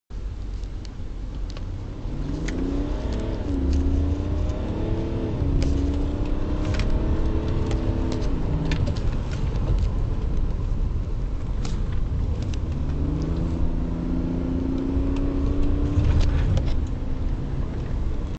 30 - 120 km/h in 7s e39 525d
While driving 30km/h I kicked down in sport mode (automatic transmission) and reached 120km/h in 7 seconds (had to slow down then cause I had no more road ahead lol)